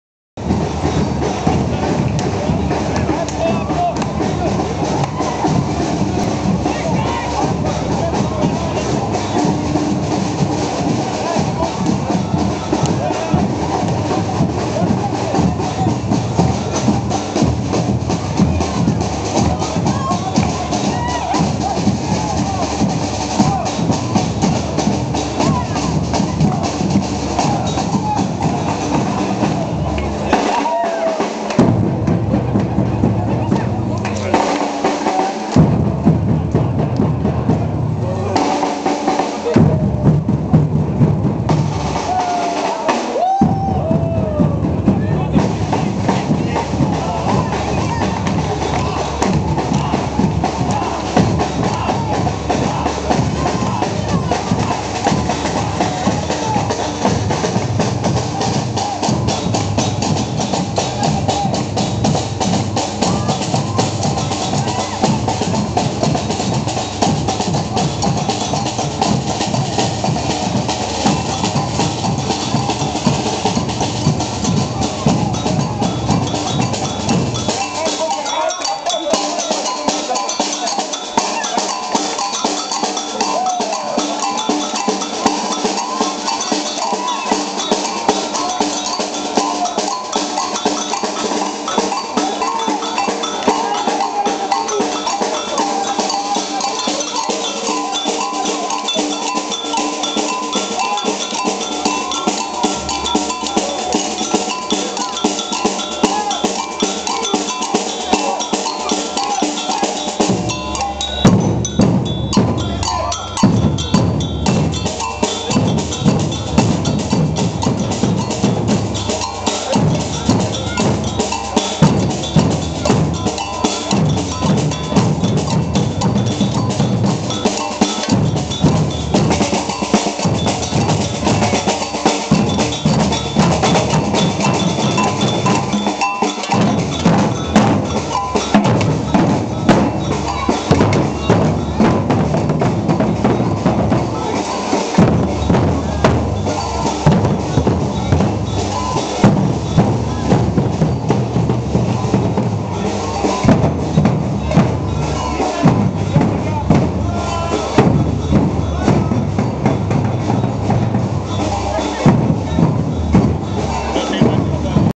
Fête du tgv le 9 juin 2007
fête à l' occasion de l' arrivée du tgv est européen à Saverne